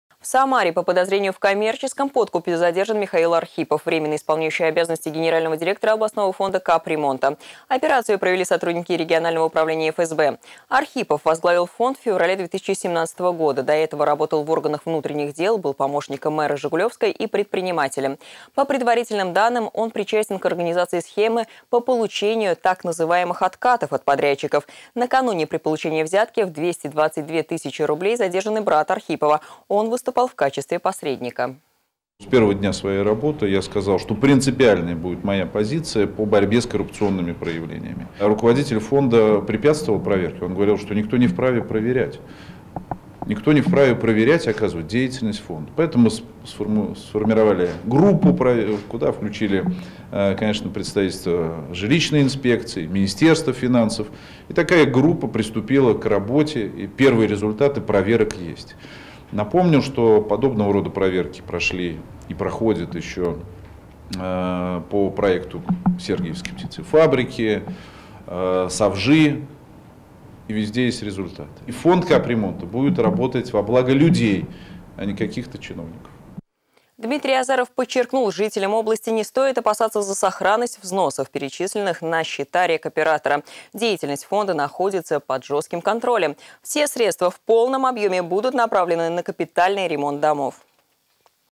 Дмитрий Азаров прокомментировал задержание главы фонда капремонта
По словам руководителя области, Михаил Архипов препятствовал проведению проверок. Однако избежать наказания не удалось.

Напомним, в Самаре по подозрению в даче взятки задержан временно исполняющий обязанности генерального директора областного фонда капремонта Михаил Архипов. Операцию провели сотрудники регионального управления ФСБ. Архипов возглавил фонд в феврале 2017 года. До этого работал в органах внутренних дел, был помощником мэра Жигулёвска и предпринимателем. По предварительным данным, он подозревается в коммерческом подкупе в размере 220 тысяч рублей. Его задержание прокомментировал глава региона Дмитрий Азаров. Он отметил, что руководитель фонда препятствовал проверке. ”Поэтому мы сформировали группу, куда включили представителей жилищной инспекции, министерства финансов, и такая группа приступила к работе. И первые результаты есть!” - отметил глава региона. Он также напомнил, что подобные проверки проходят по проекту Сергиевской птицефабрики, СОФЖИ. ”Фонд капремонта будет работат во благо людей, а не каких-то чиновников”, - подытожил Дмитрий Азаров.